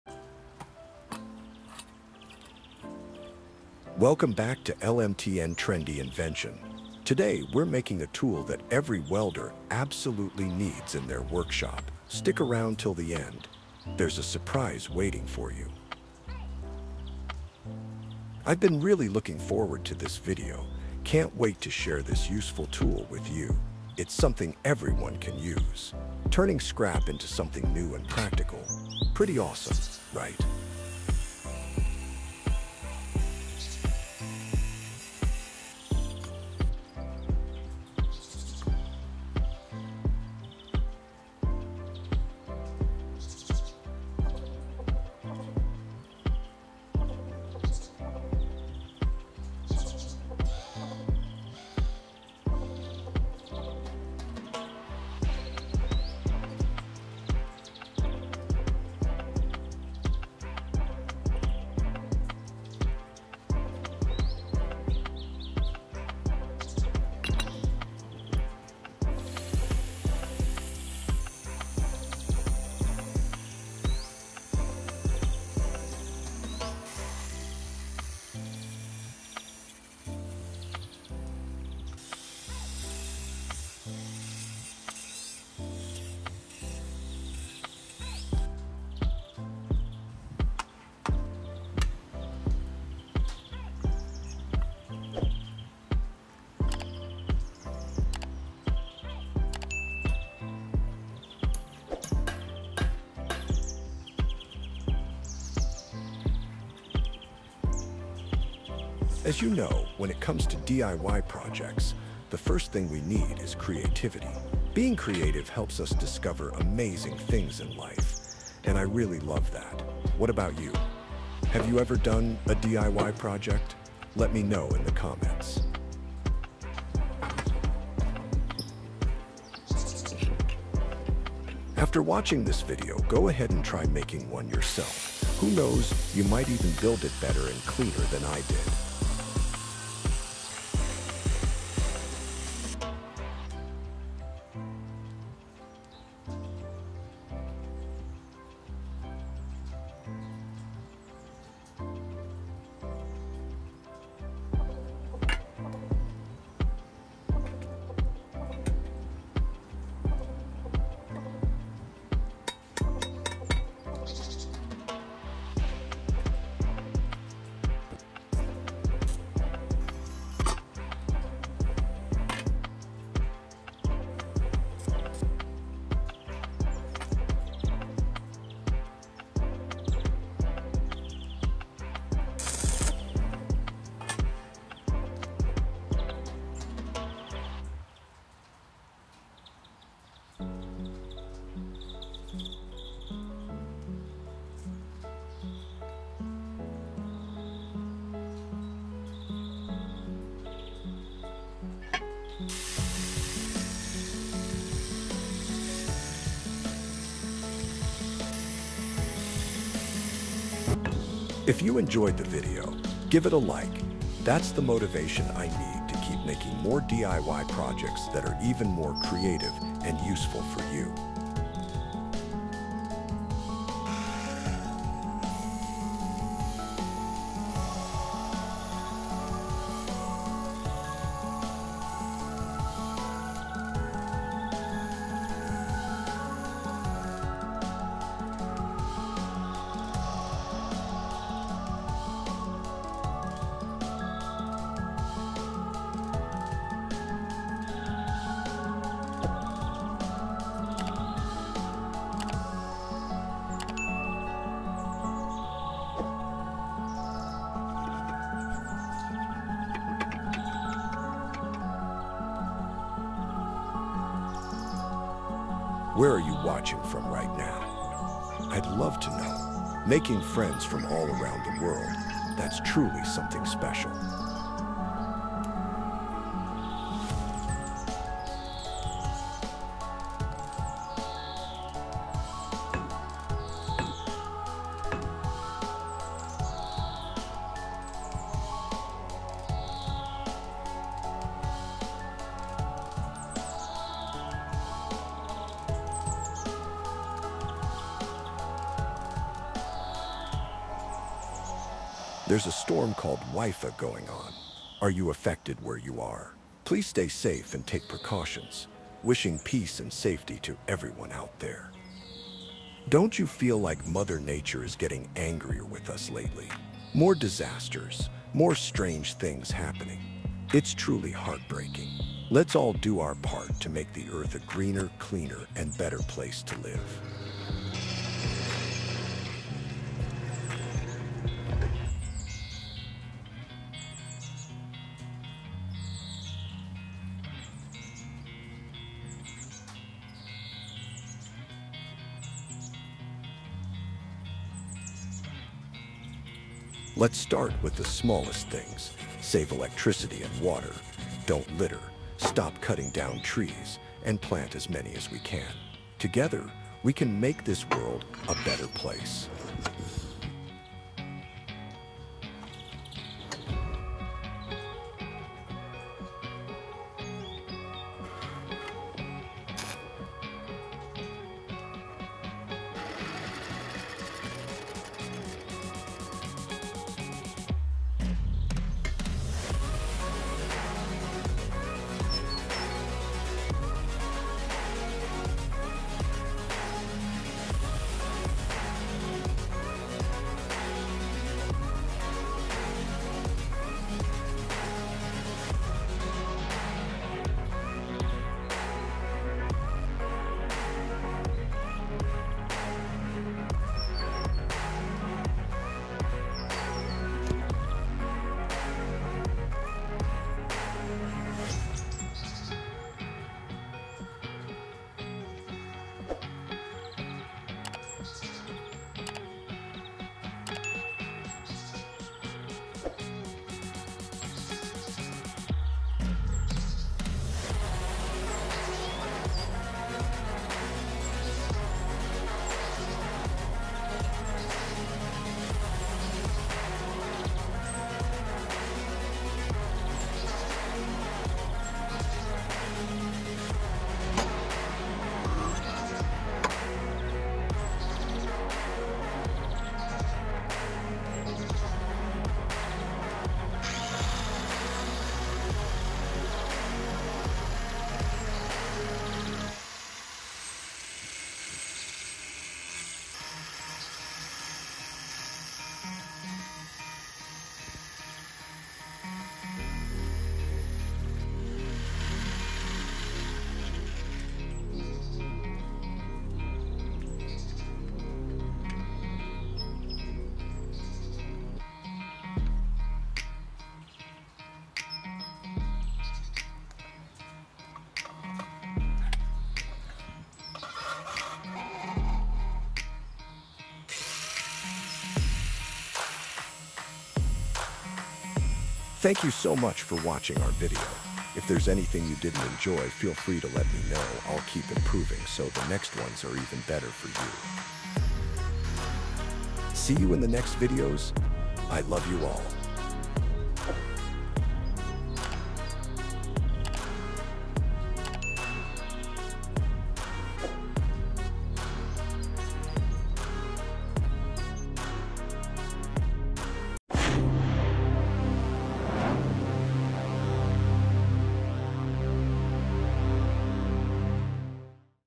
Unique Uses of Bearings in DIY Projects – Super Clever!
🌀 Did you know that bearings can do way more than just spin wheels?
In this video, we reveal unique and clever uses of bearings in DIY projects that most people have never seen!

🛠️ From building homemade tools to creative mechanical tricks, you'll learn how to repurpose bearings from old machines and scrap metal into something truly useful.
These are the kinds of DIY bearing hacks that turn simple ideas into powerful solutions for your garage, farm, or workshop.

✨ Perfect for anyone who loves:

🔧 DIY engineering projects

⚙️ Scrap metal builds

🧰 Creative tool hacks using bearings

💡 Whether you're a beginner or a serious maker, these ideas will inspire your next project.

💬 Have you used bearings in a clever way? Share your ideas in the comments!